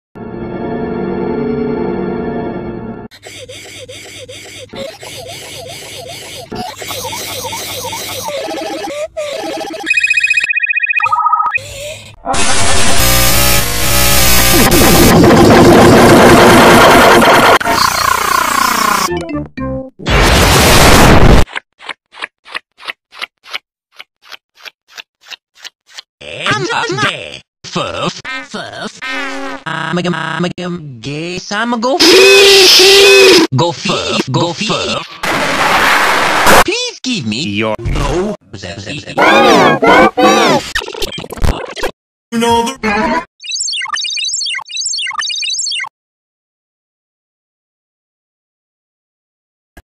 (YTP TENNIS) No, The Fluroscence and hayla needs to stop
SylveonXD555's round 3 vs CYRYUSBOI02
Originally reuploaded by CYRYUSBOI02, but the original reuploader has been hacked by a scammer